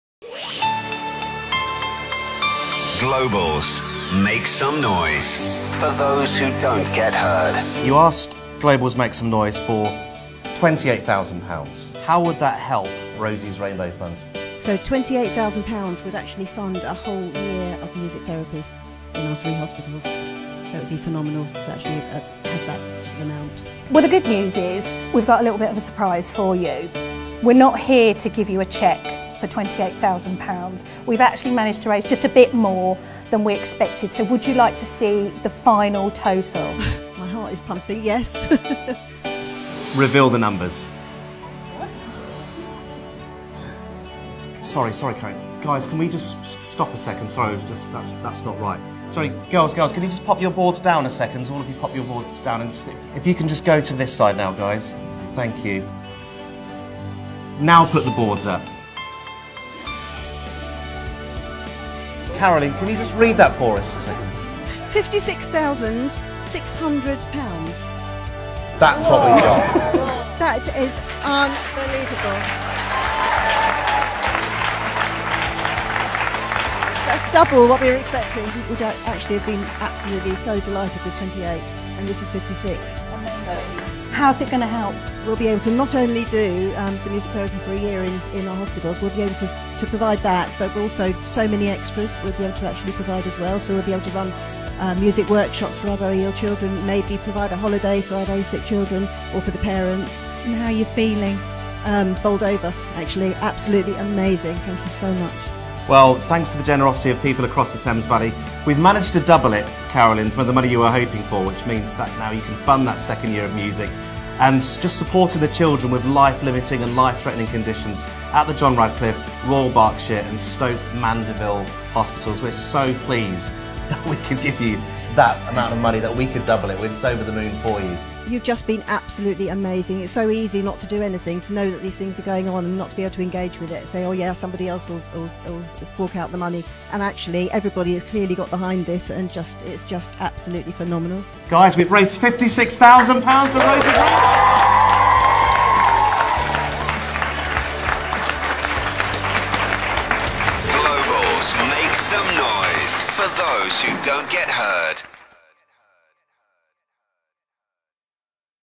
Rosie's Rainbow Fund Receives £56,600 Grant from Global's Make Some Noise
Rosie's Rainbow Fund was recently surprised at Rosie's family's school, Redroofs School for the Performing Arts, by Matt and Michelle from Heart Thames Valley. Matt and Michelle revealed the total raised for Rosie's Rainbow Fund through the Global's Make Some Noise campaign--an astonishing £56,600 to fund two days of music therapy work at the John Radcliffe Children's Hospital in Oxford for two full years! We are thrilled, bowled over, delighted and excited. Hugest thanks to Heart Thames Valley, Global's Make Some Noise, Ross Beresford and the Heart team, Matt and Michelle, Hannah, Kara, Hannah, and all the team at Global's Make Some Noise, and to all who donated and supported Global's Make Some Noise! We are truly grateful :-)